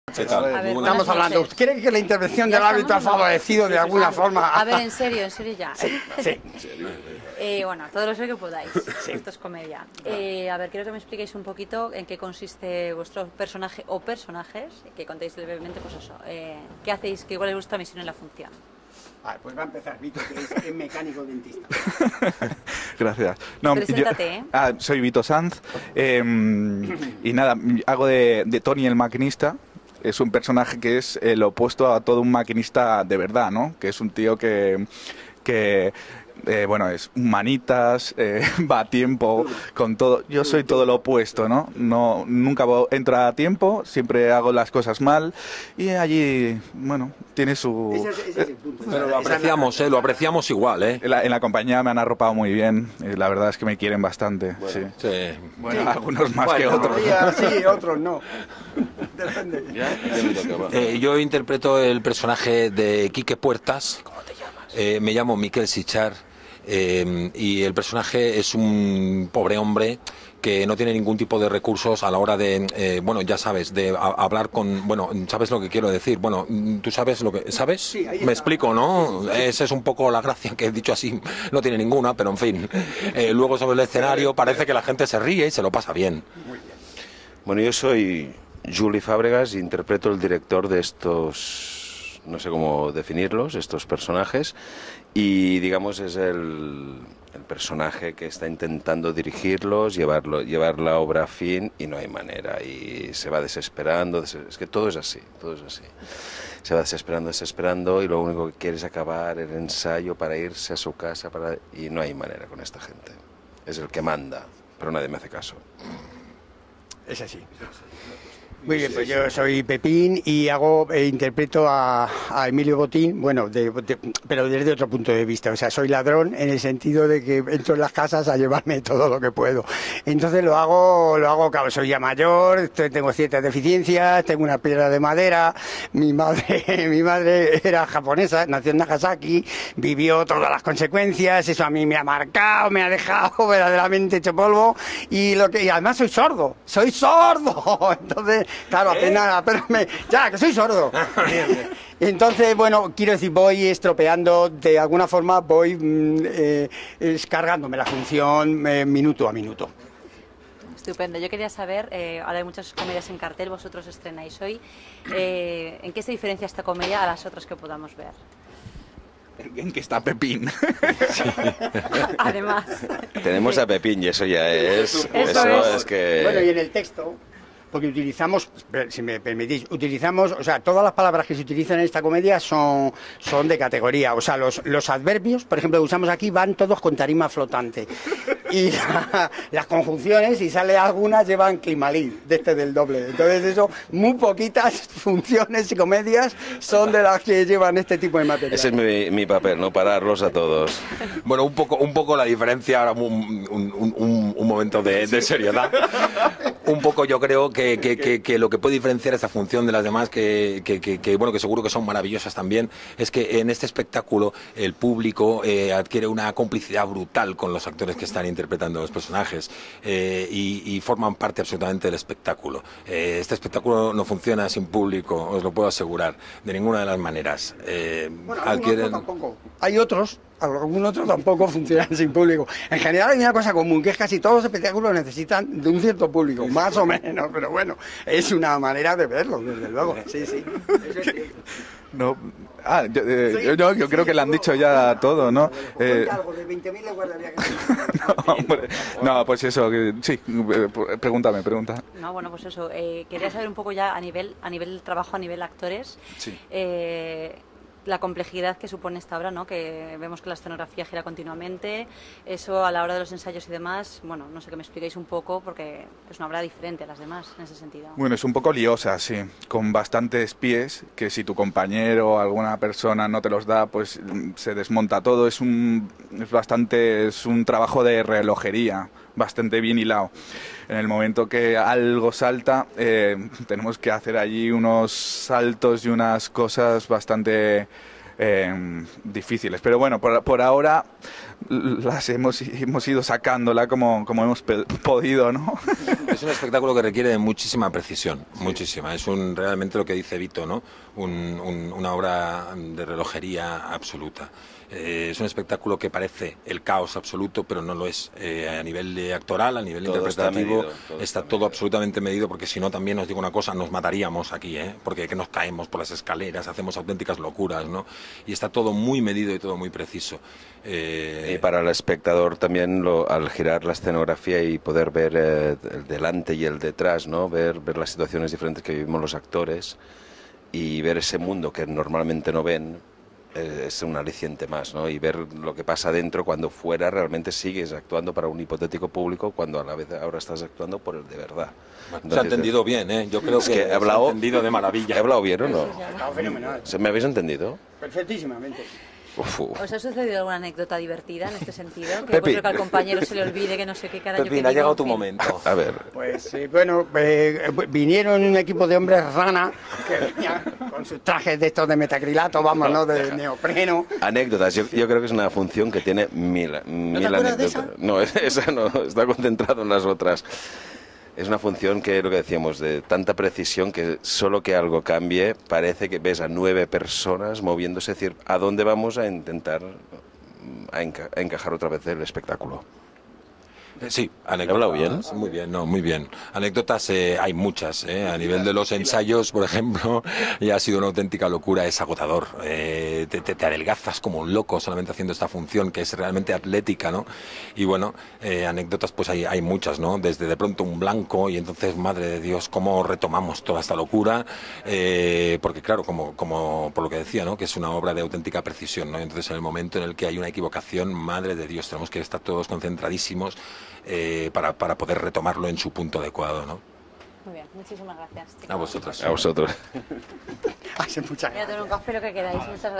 ¡QUÉ DESASTRE DE FUNCIÓN! - Entrevista Vito Sanz, Miquel Sitjar, Pepin Tre y Juli Fabregas
TEATRO CASER CALDERÓN. "¡Que desastre de función! (por delante y por detrás)" podrá verse en el teatro Cáser Calderón desde el 31 de octubre de 2013 de miércoles a viernes a las 20.00h; los sábados a las 17.00h y 20.00h; los domingos a las 18.30h. Las entradas pueden adquirirse de 15 a 40 euros.
¡Qué desastre de función! se estrenó en Londres en el año 1982 y desde entonces se ha estado representando continuamente en algún lugar del mundo, se ha traducido a 28 idiomas y se ha representado en más de 50 países. Incluso se ha hecho una adaptación al cine titulada "Noises Off" que dirigió Peter Bogdanovich y que protagonizaron, entre otros, Michael Caine, Christopher Reeve y Carol Burnett. El espectador no espera ver dos comedias en una: la primera, un vodevil bastante ordinario. La segunda muestra las dificultades de todo tipo que sufre la compañía para montar y girar esta magnífica obra. Pero, esto no es todo. El autor nos reserva una sorpresa que comienza en el segundo actor cuando el decorado comienza a girar para dejarnos ver la farsa delirante que tiene lugar entre bambalinas: actores y actrices peleándose en silencio mientras entran y salen del escenario para seguir la función como si adentro no se estuvieran matando.